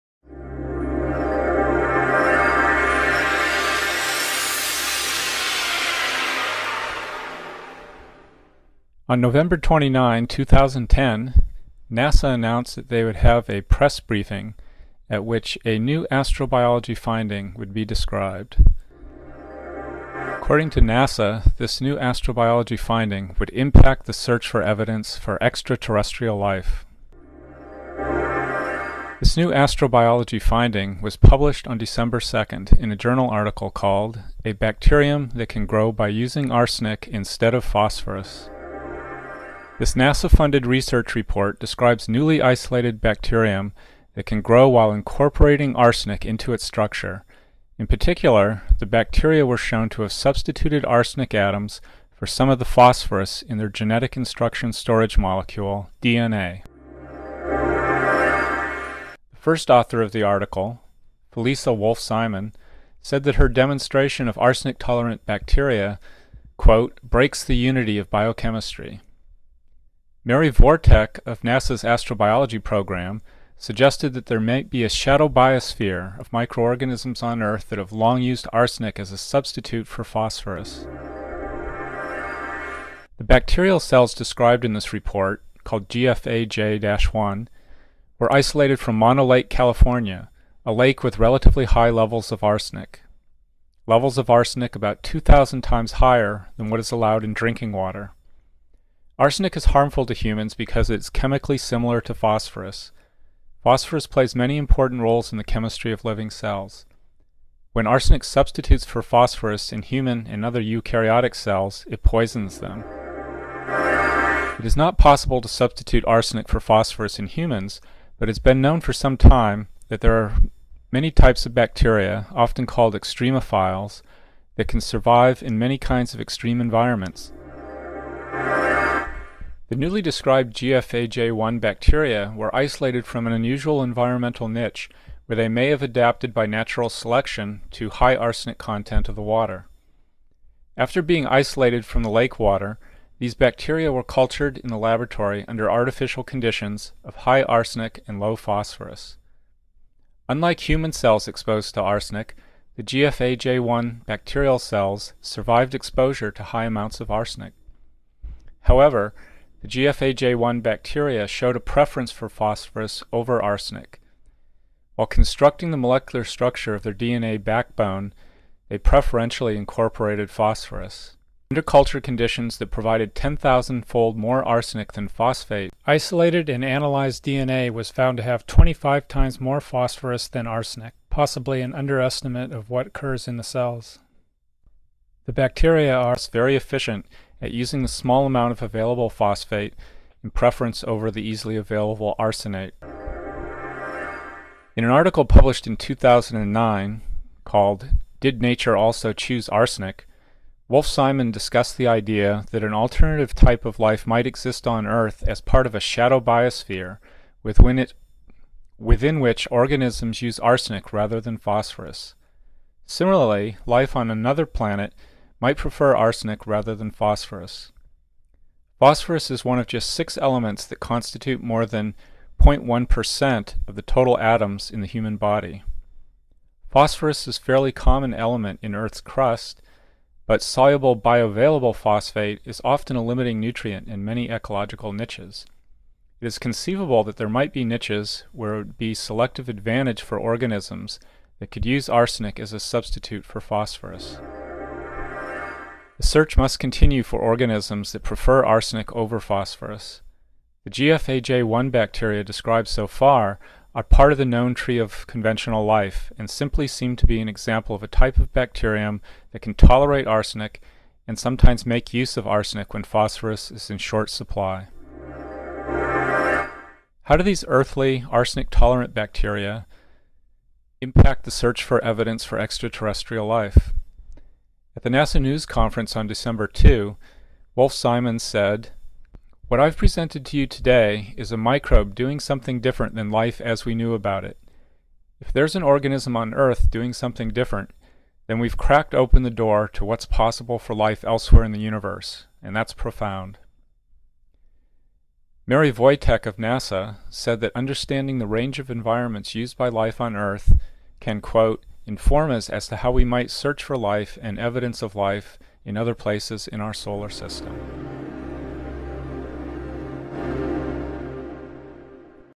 Replacement of phosphorus by arsenic in GFAJ bacteria
Replacement of phosphorus by arsenic in living organisms. Description of recent results showing that some bacteria can tolerate arsenic and incorporate it into their DNA.